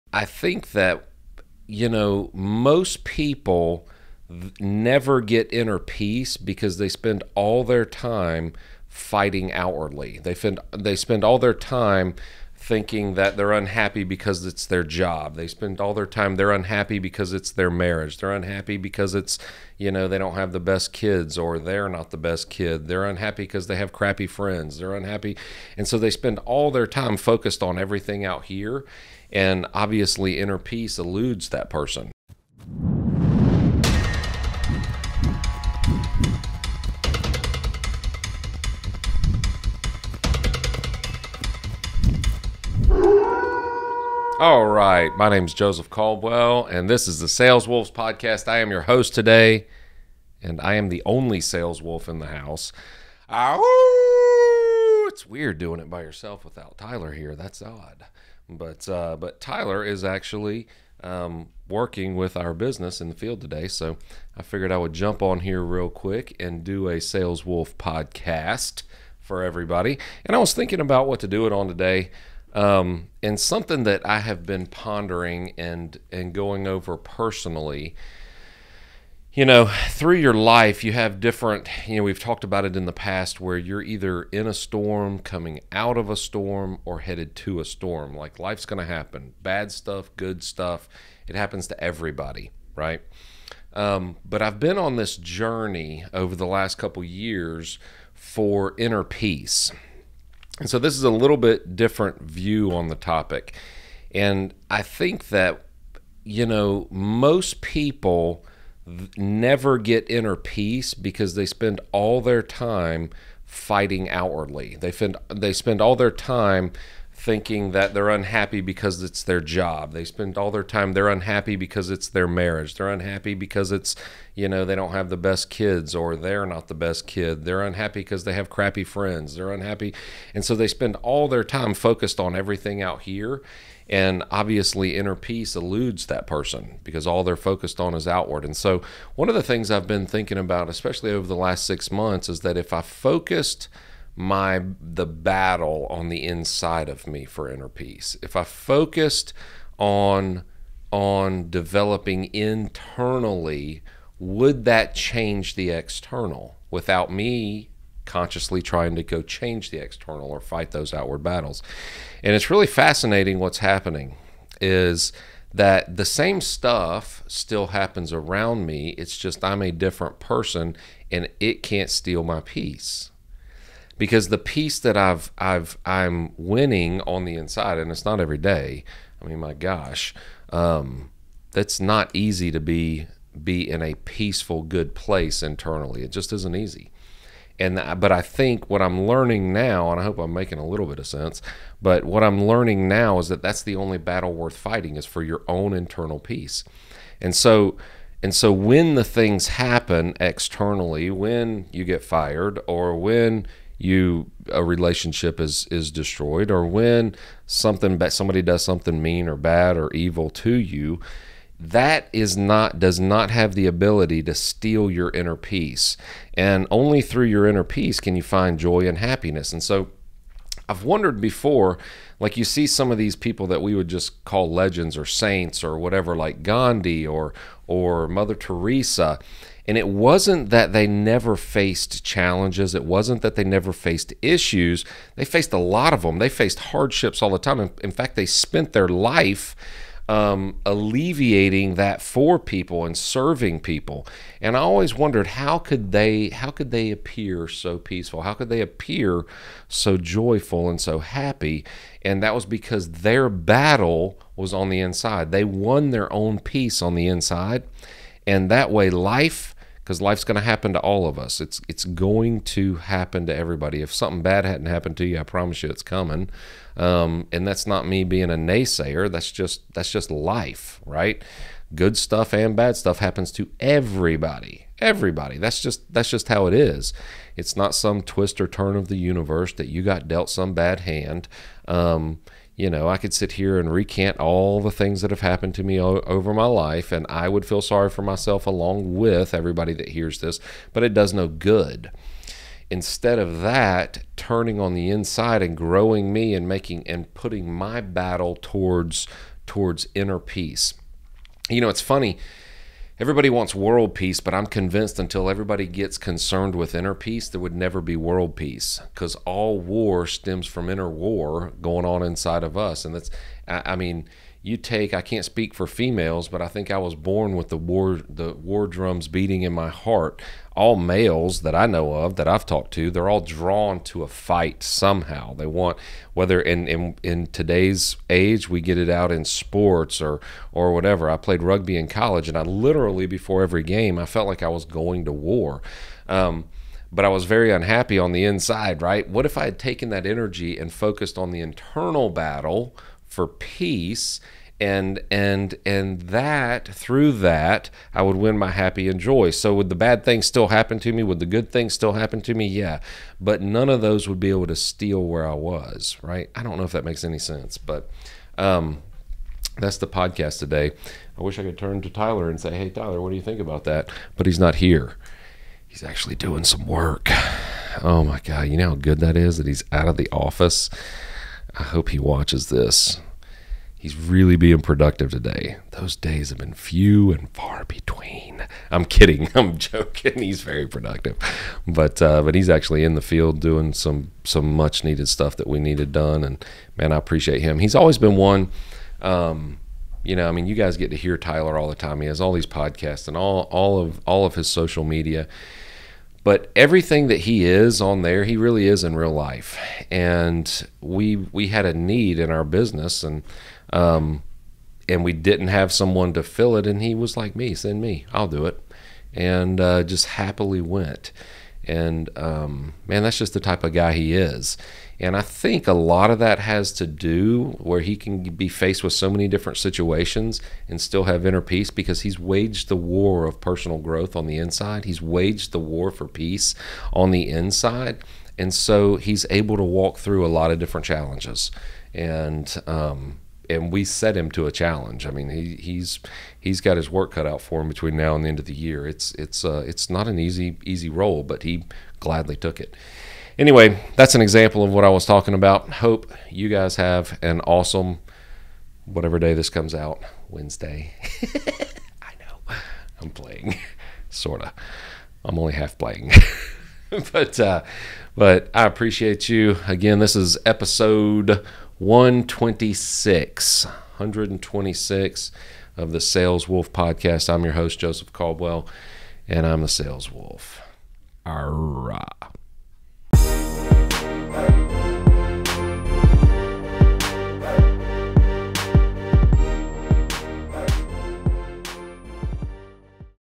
Sales Wolves Podcast | Episode 126 | Inner Peace
Learn to have inner peace. It doesn't mean that outward problems won't still arise, but having inner peace is something you can control, so the outward problems can't harm you and your peace.